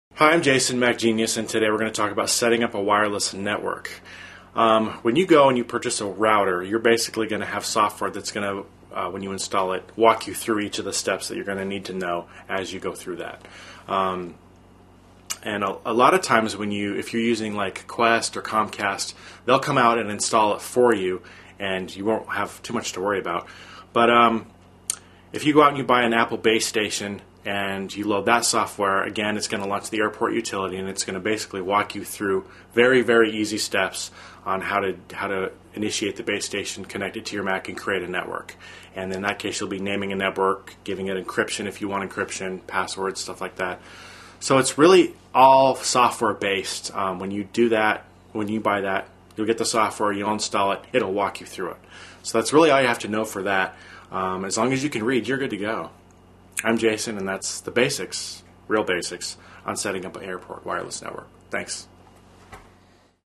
Computer Network Tips : How to Set Up a Wireless Network
Setting up a wireless computer network involves using the set up software that comes with a computer or by following the instructions on the computer itself. Connect to a wireless computer network with tips from a computer specialist in this free video on computer networking.

Expert: Jason Hendricks
Bio: Jason Hendricks is a Macintosh genius who works for the Mac Store in Denver, Colo.